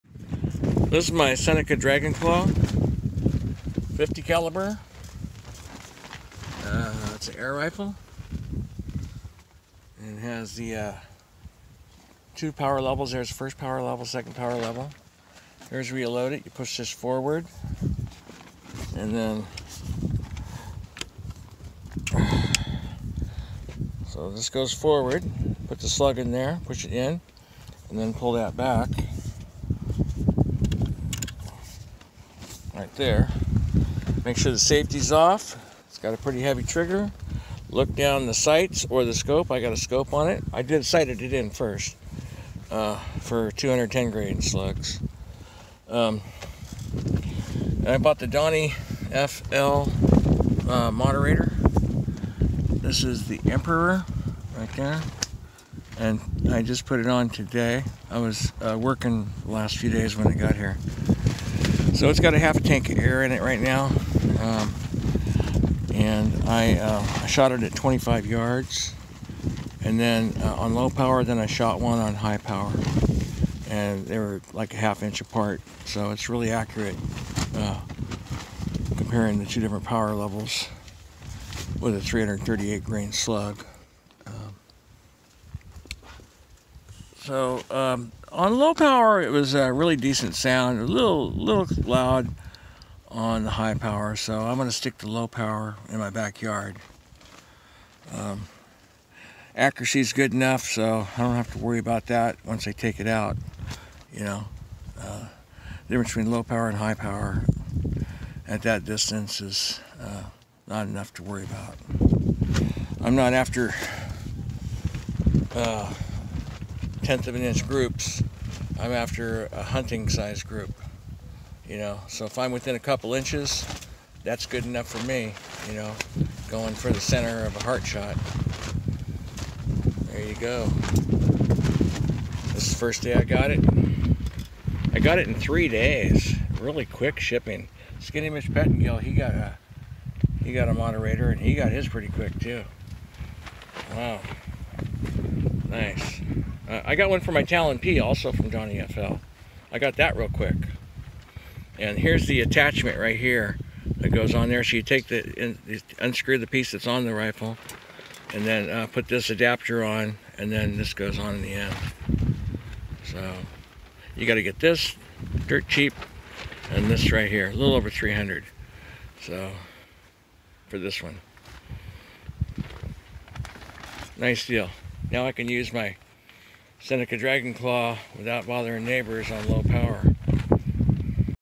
Seneca Dragonclaw with the DonnyFL Moderator
I mention that on low power the sound is pretty low.  On high power the sound is a bit louder